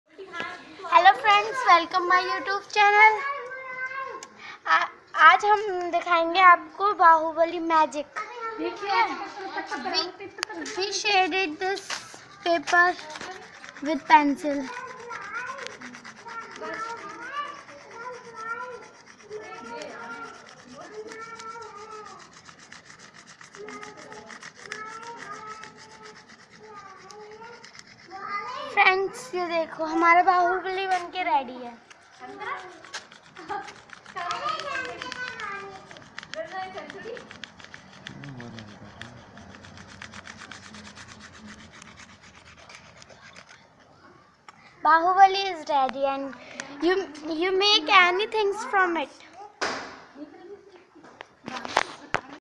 HOW TO Make Bahubali Scene In Hindi
नमस्कार दोस्तों यह वीडियो मैजिक बुक के बारे में  है  जब बुक के पेज पर पेन्सिल  चलते है तो उसपर एक कार्टून बन जाता है बहुत कम समय में बन जाता है और बुक में बहुत पेज होते है मैजिक करने के